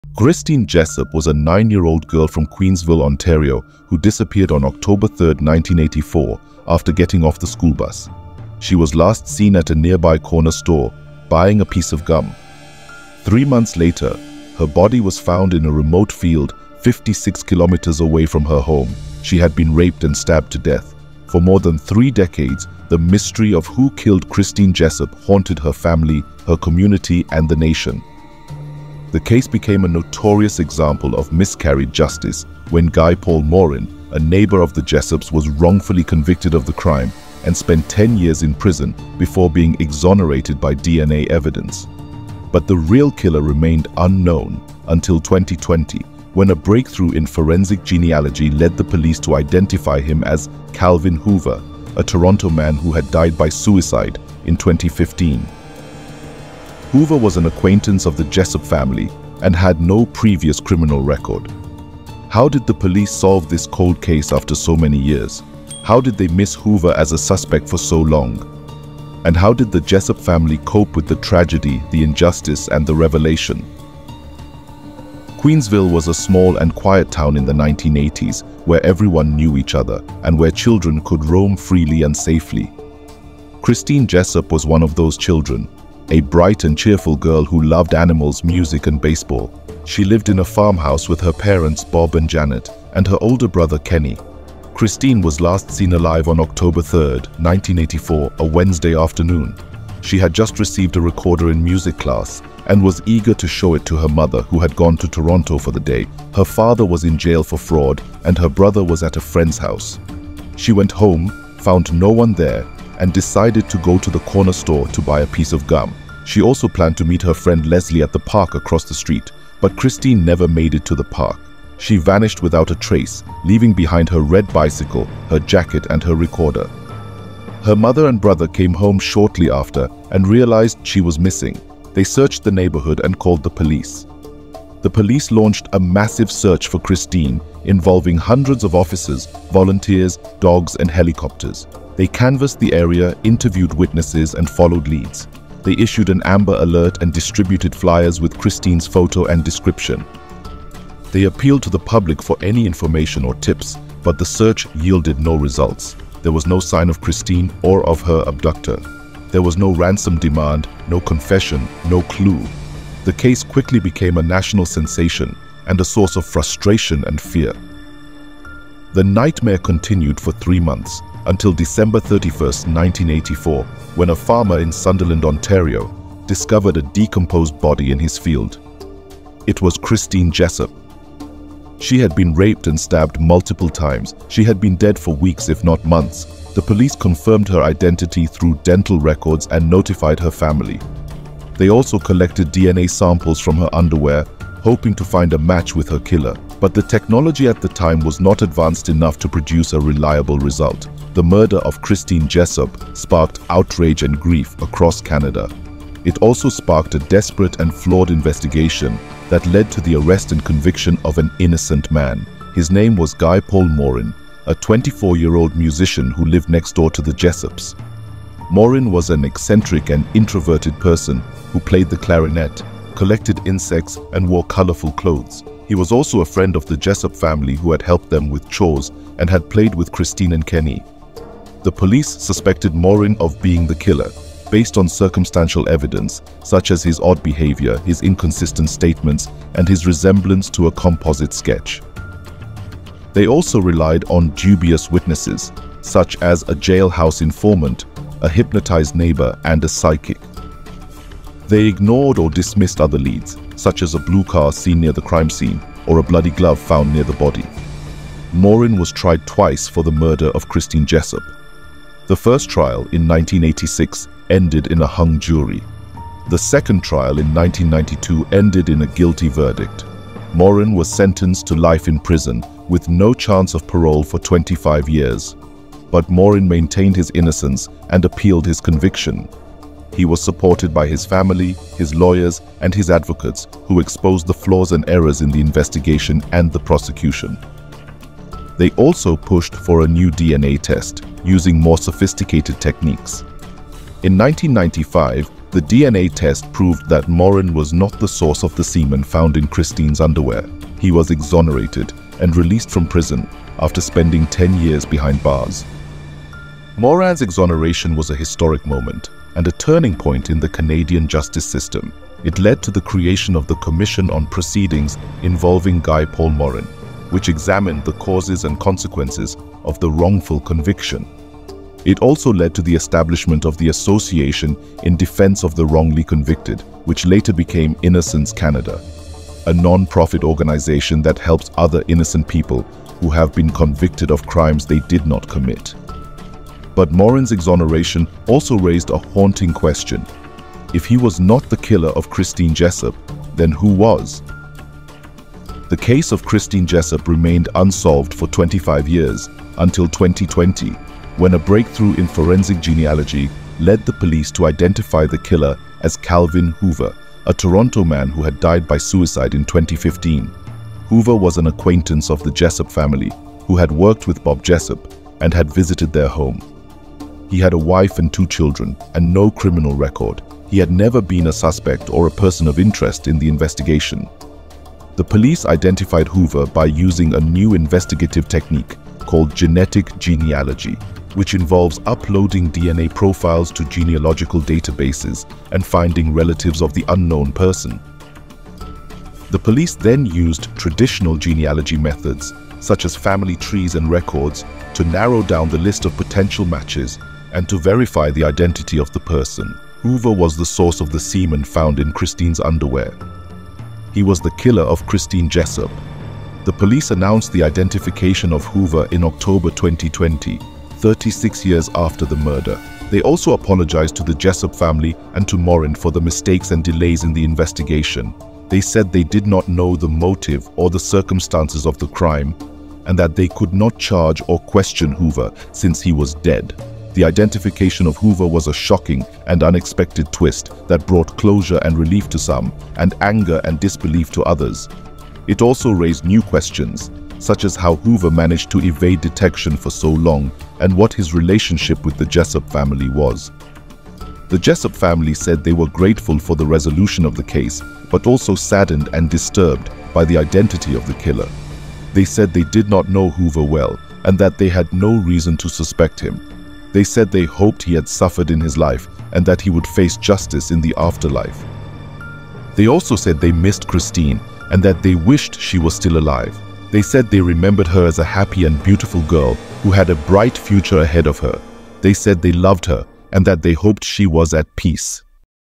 How Police Identified Christine Jessop’s Rapist and Killer
In this video, we explore the shocking story of Christine Jessop, a nine-year-old girl who was abducted, raped, and murdered in 1984. We reveal how police used a new forensic technique called genetic genealogy to identify her killer, Calvin Hoover, 35 years after the crime. We also examine the wrongful conviction of Guy Paul Morin, who spent 18 months in prison for a murder he did not commit. How did police finally solve this cold case? What are the ethical implications of using genetic genealogy to track down criminals? Watch this video to find out.